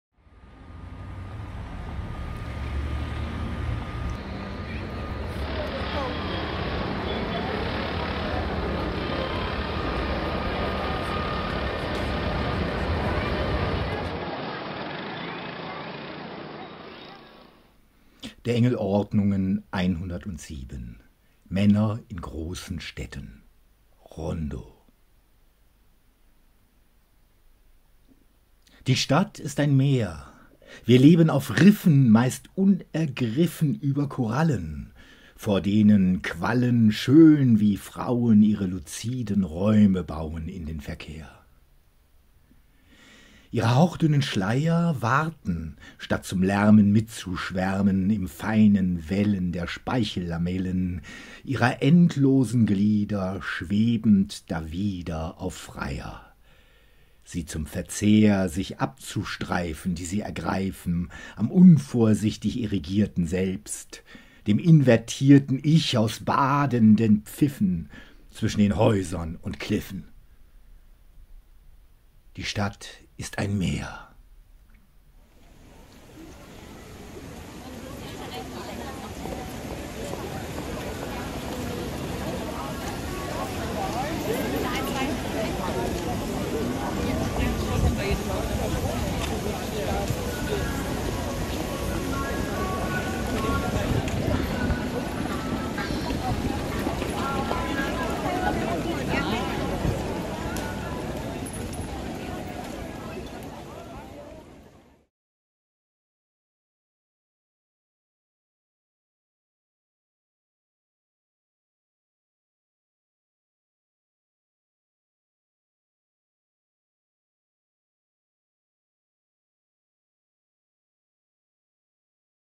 Alban Nikolai Herbst " Männer in großen Städten / Rondo "
aus Alban Nikolai Herbst, "Der Engel Ordnungen", Gedichte, 2008.

In Zeiten Covid-19s jeden Tag ein Gedicht. Zweite Serie, einhundertsiebterTag.

Ein Flaneurgedicht.